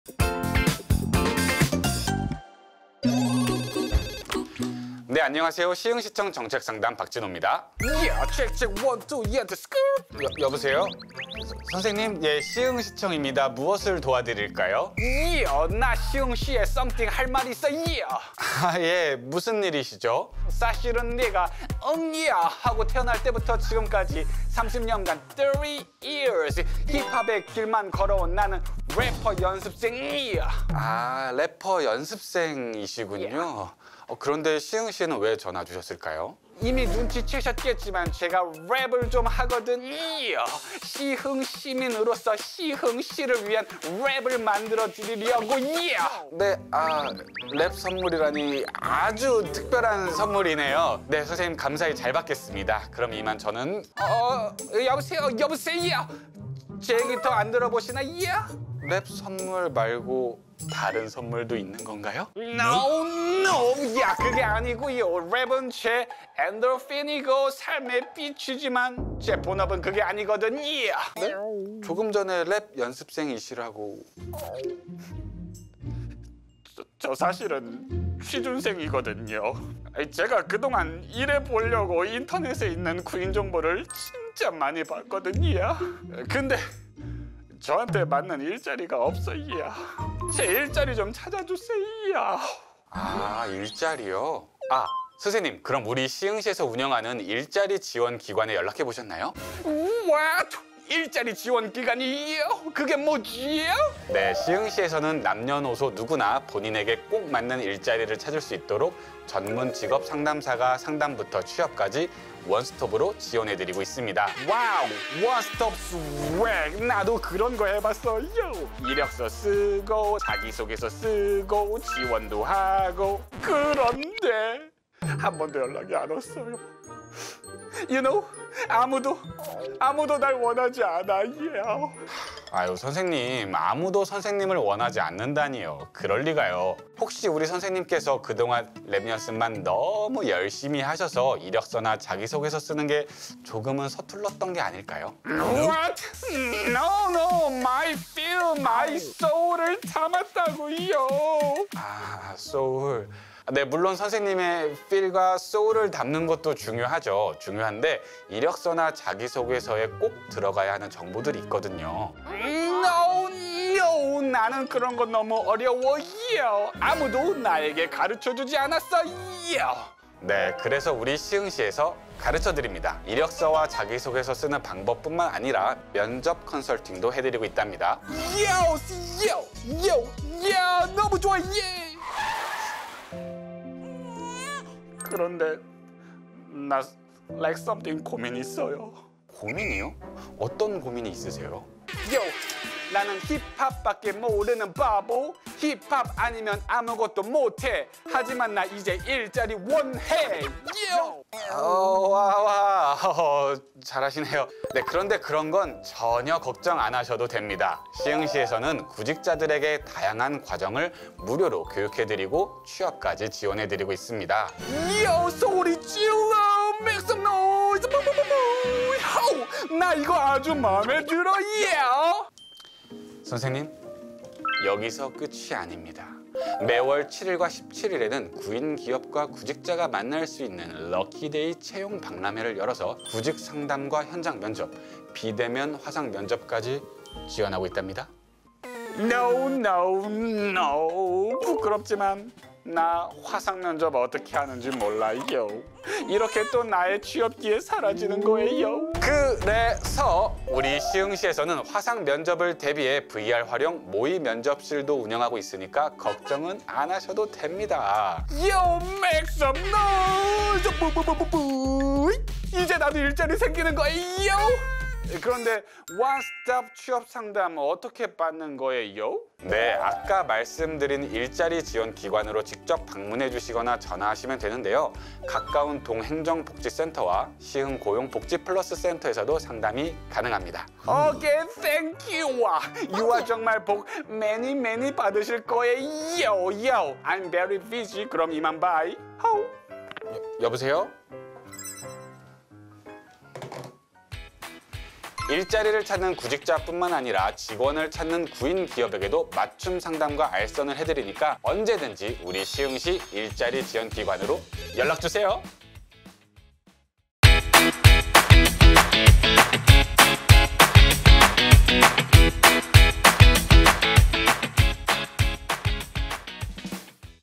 [시흥은정책ez 시즌4] 시흥에서 일자리 찾기 어렵지 않아요!
취업준비, 시흥시와 함께라면 어렵지 않~아요~~
시흥시 일자리 지원기관에서 여러분의 취업을 도와드립니다!